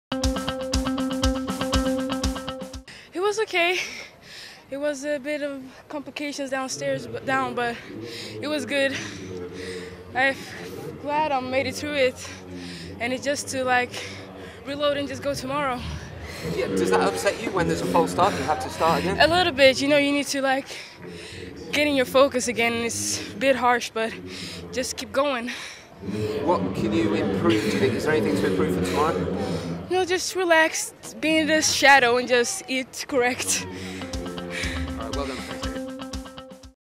WYC Donetsk 2013 - Adriana Janic SWE
IAAF World  Youth Championships Donetsk 2013 Ukraine
Adriana Janic SWE
100m Hurdles Heats